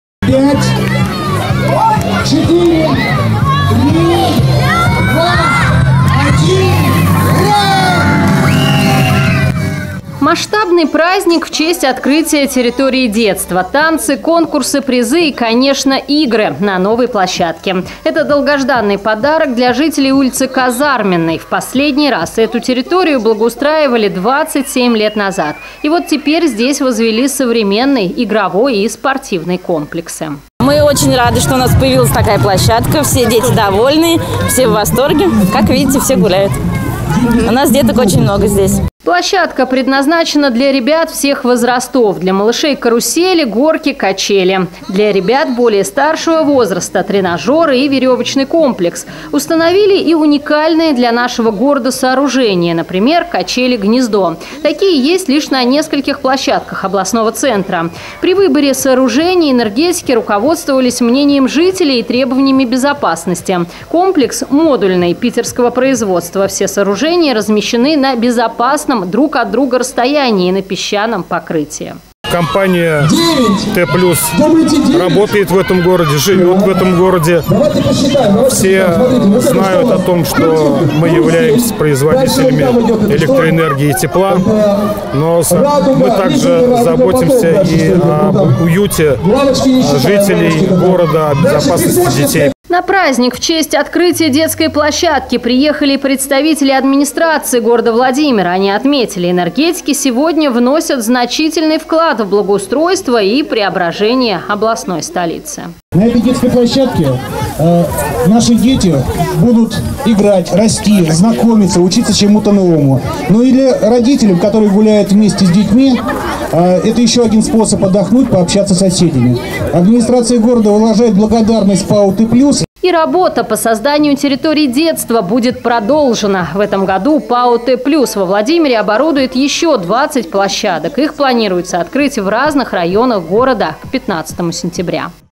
Новая детская площадка
Во Владимире на улице Казарменной в районе дома №5а открыли новую детскую площадку.

Установила её компания ПАО "Т Плюс". Вот уже третий год энергетики в рамках корпоративной программы благоустраивают дворы областного центра.